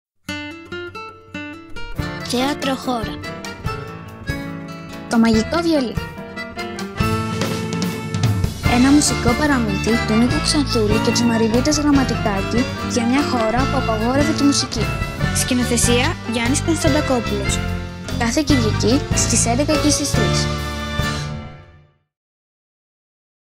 Το "Μαγικό Βιολί" στο Θέατρο Χώρα - Trailer
Η παιδική σκηνή του Θεάτρου Χώρα υποδέχεται την θεατρική περίοδο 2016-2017 «Το Μαγικό Βιολί» του Νίκου Ξανθούλη και της Μαριβίτας Γραμματικάκη.
Στη χώρα που απαγορεύονται η μουσική και τα χρώματα, η πραγματικότητα φοράει τα ρούχα του παραμυθιού για να φέρει στους μικρούς θεατές και στα αιώνια παιδιά την αλήθεια της μουσικής, της χαράς και της αισιοδοξίας.

Σκηνοθεσία: Γιάννης Κωνσταντακόπουλος
Σχεδιασμός και εικαστική επιμέλεια σκηνικού χώρου: Μυρτώ Λάμπρου
Κοστούμια: Ηλένια Δουλαδίρη
Φωτισμοί: Κατερίνα Μαραγκουδάκη
Επιμέλεια Κίνησης: Έλενα Γεροδήμου
Video Art & 3D Animation: Σπύρος Κουδουνάς
Εικονογράφηση: Μαρίνα Δημοπούλου
Βοηθός Σκηνοθέτη: Ελένη Κάκκαλου
Βοηθός ενδυματολόγου: Μαίρη Μαρμαρινού
Βοηθός χορογράφου: Μαρία Γεροδήμου
Φωτογραφίες - Βίντεο: Μαριλένα Αναστασιάδου / ICON Productions
Προβολή – Επικοινωνία: BrainCo S.A.
Παραγωγή: Purple Group

ΠΑΙΖΟΥΝ ΟΙ ΗΘΟΠΟΙΟΙ (Αλφαβητικά):
Γρηγόρης Γεωργίου, Φένια Ζαχαρίου, Ελένη Κάκκαλου, Ιωάννης Κοντέλλης, Δημήτρης Κυρατσούδης, Ηρακλής Κωστάκης, Τζωρτζίνα Παλαιοθόδωρου, Νικόλας Στραβοπόδης, Γιώργος Τζαβάρας

Έναρξη παραστάσεων: Κυριακή 25 Σεπτεμβρίου
Παραστάσεις: Κάθε Κυριακή,  11:00 & 15:00
Καθημερινά οργανωμένες παραστάσεις για σχολεία

Θέατρο Χώρα
Αμοργού 20, Αθήνα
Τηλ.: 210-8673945